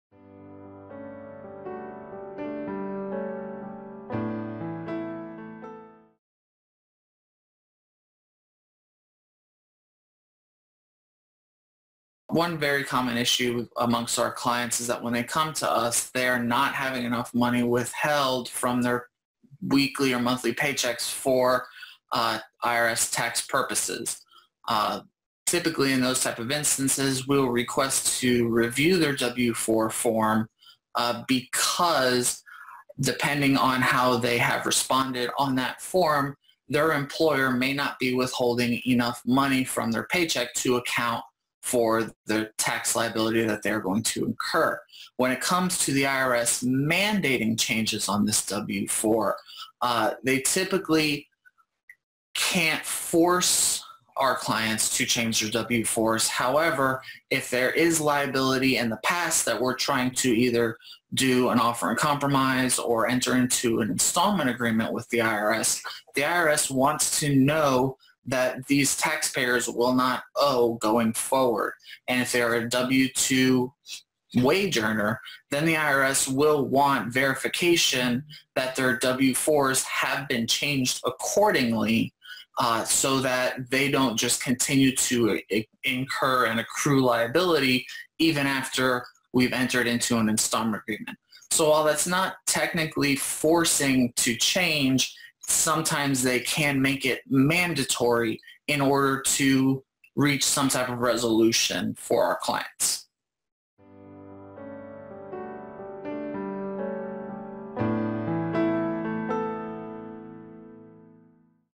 Can The IRS Force Me To Change The Number Of Dependents On My W-4 Form?
The Greene Law Firm, P.A.
11 McGee Street
Greenville, SC 29601
United States

The IRS can't force you to change the number of dependents on your W-4 form but sometimes the IRS can make it mandatory in order to reach some type of resolution.